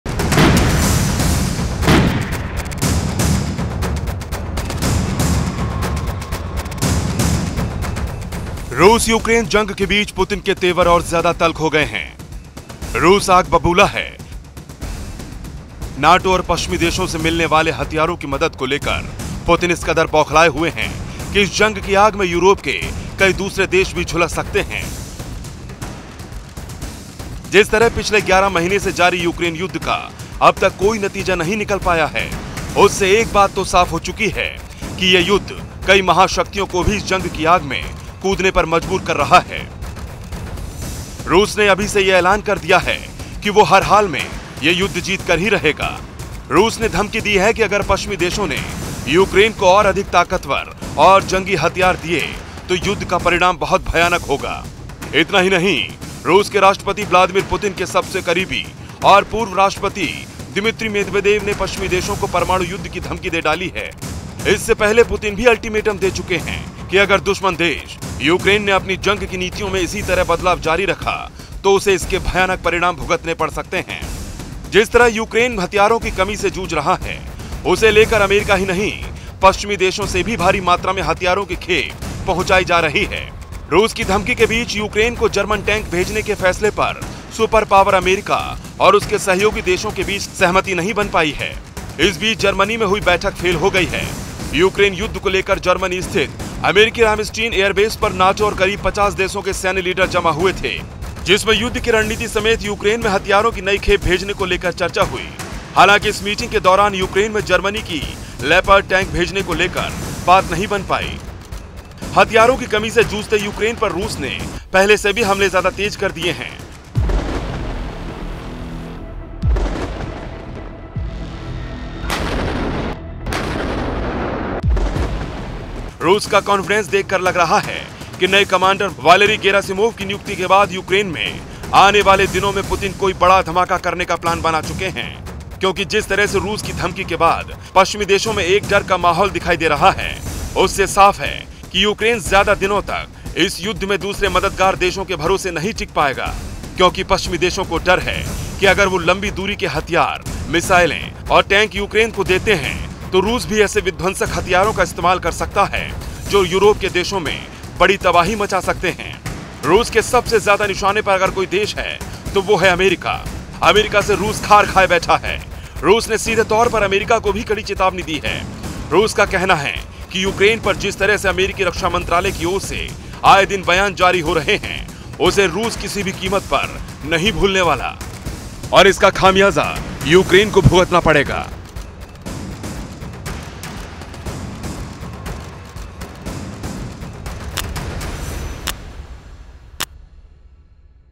Putin War Plan: रूस गिराने वाला है यूक्रेन पर परमाणु बम? पुतिन के करीबी ने जो कहा, सुनकर हिली दुनिया
रूस-यूक्रेन जंग के बीच पुतिन के तेवर और ज्यादा तल्ख हो गए हैं. यूक्रेन को Nato और पश्चिमी देशों से मिलने वाले हथियारों की मदद को लेकर पुतिन बौखलाए हुए हैं. रूस ने धमकी दी है कि अगर पश्चिमी देशों ने यूक्रेन को और अधिक ताकतवर और जंगी हथियार दिए तो युद्ध का परिणाम बहुत भयानक होगा. हाल ही में रूस के राष्ट्रपति vladimir putin के सबसे करीबी dmitry medvedev ने पश्चिमी देशों को परमाणु युद्ध की धमकी तक दे डाली.